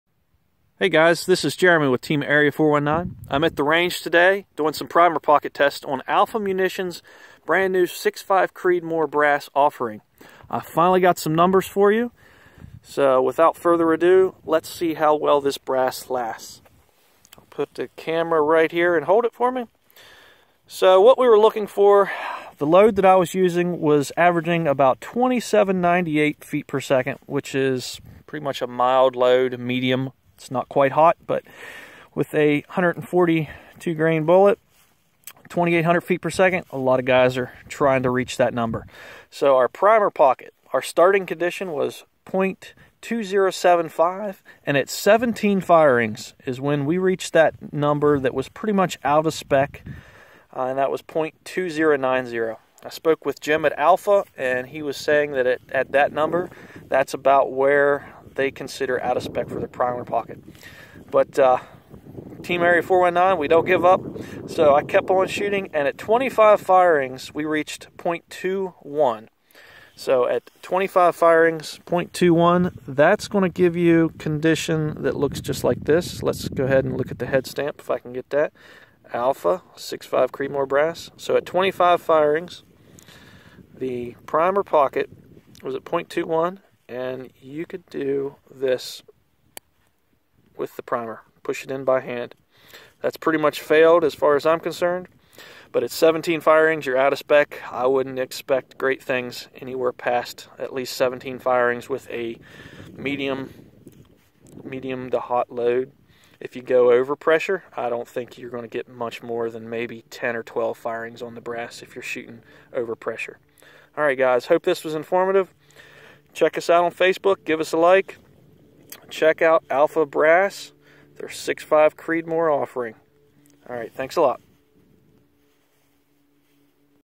Alpha 6.5 Creedmoor brass torture test
Keep in mind that everyone has the potential for different results due to variables in rifle systems and ammo components.

That being said...Here's the Alpha Munitions 6.5 Creedmoor pocket test video results as we experienced them. The short story is that Alpha Munitions is a contender and you need to keep an eye on them!

Test went like this.
One, randomly selected, piece of brass was shot, reloaded and shot again through the course of this test. The same powder charge, powder type, bullet and primer type was used throughout. Brass was full-length sized with a bushing each time, bumping the shoulder back about 0.002". No other work was done to the brass throughout the test.

Some numbers:
Initial primer pocket diameter: 0.2075"
Initial case head diameter: 0.4710"
Initial overall length: 1.907"

Primer pocket diameter after:
10 firings: 0.2075"
15 firings: 0.2085"
17 firings: 0.2090"
20 firings: 0.2095"
25 firings: 0.2100"

Case head diameter after 25 firings: 0.4745"
Overall length after 25 firings: 1.939"

The neck did not crack and the seating of the bullet still felt very smooth and consistent each time.

A good indicator that this brass has reached its lifespan will be the difficulty getting it into your shell holder as the case head expands...you may not notice this using the Co-Ax as quickly though.